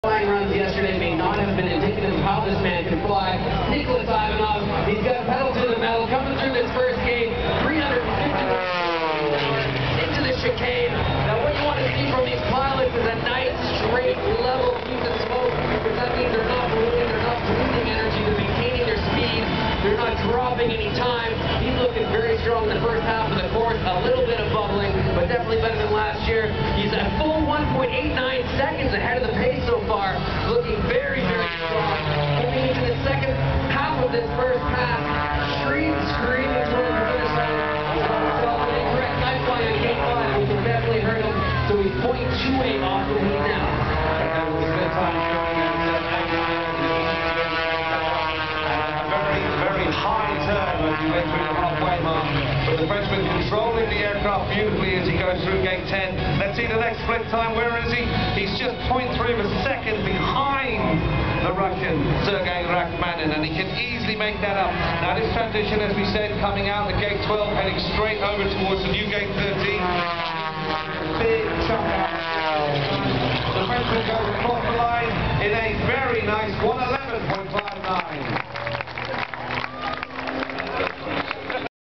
Red Bull Air Race Windsor/Detroit 2010 - Nicolas Ivanoff
French Pilot, Nicolas Ivanoff in the 1st round top 12 race.